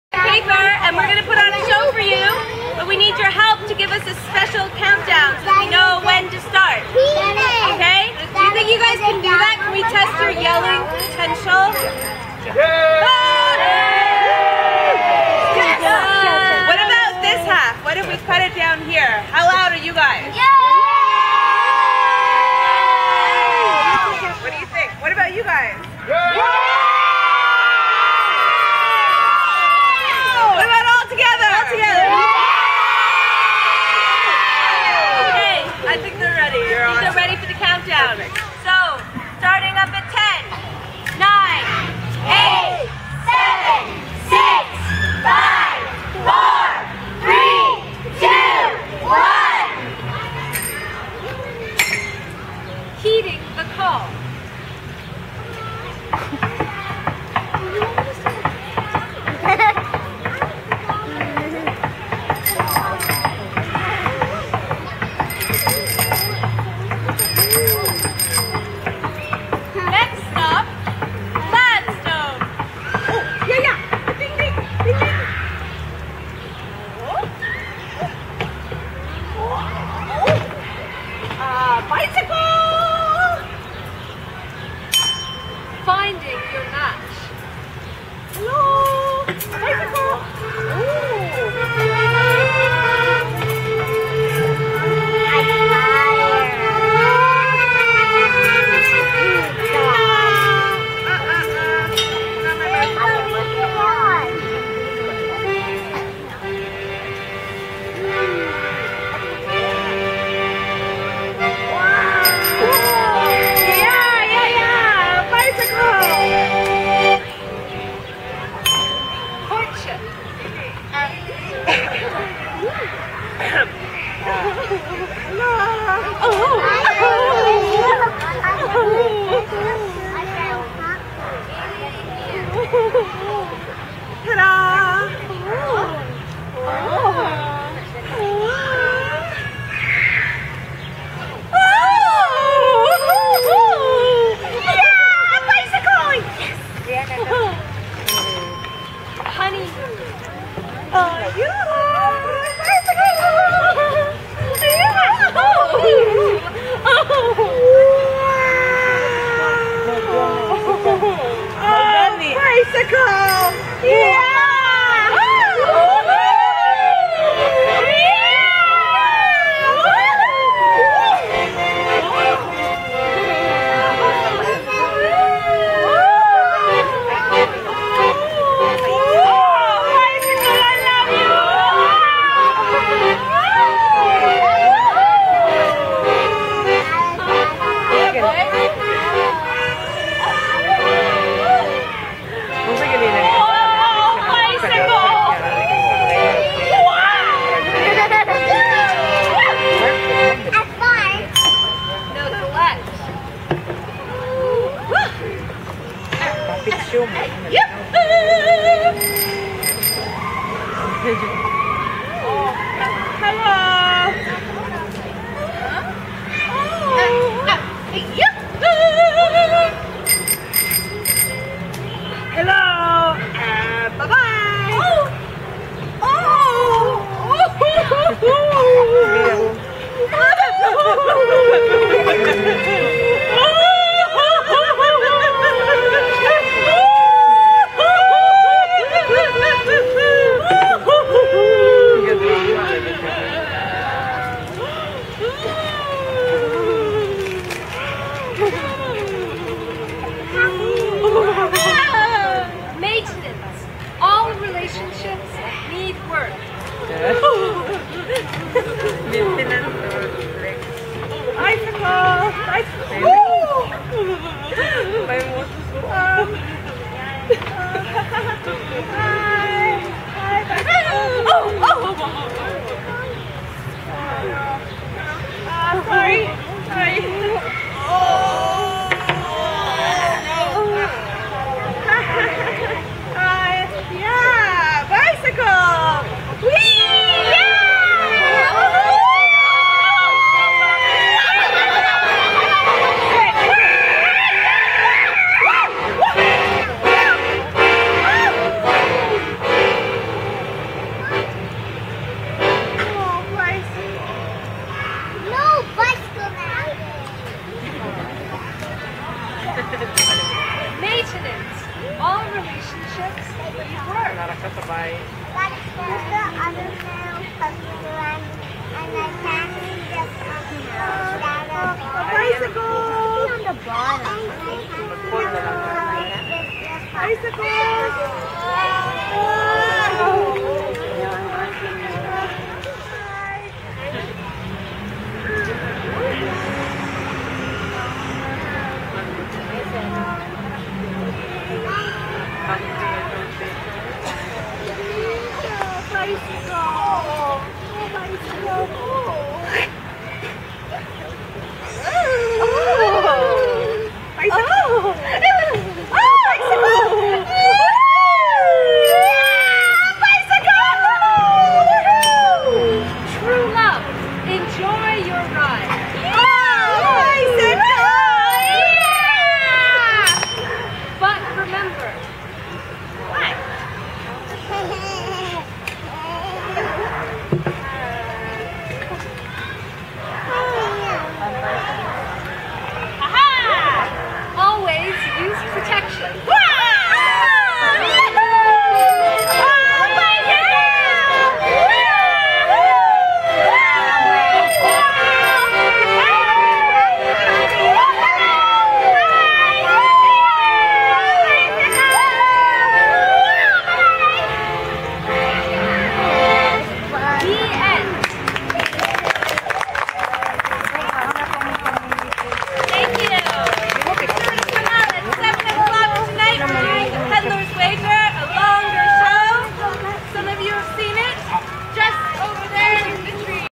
CYCLOPS: Human & Bicycle
A quick showing of the Puppet Squad's short puppet play, "Human & Bicycle, A Love Cycle", August 2011.